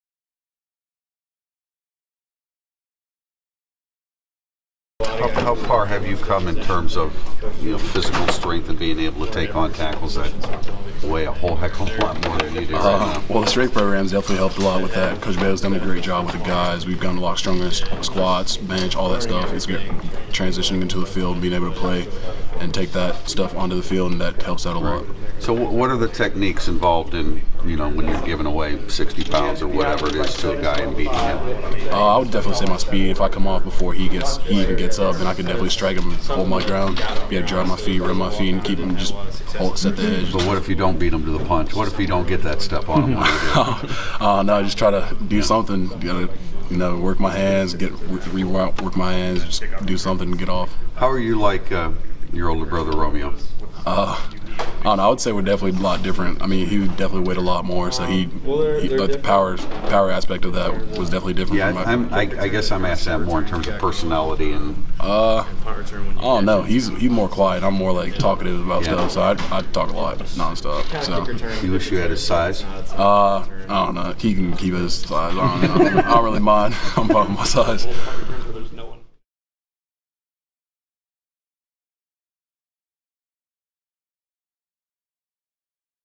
Julian Okwara on what makes him different than his brother
Julian Okwara talks about his game and what makes him different than his NFL brother Romeo Okwara.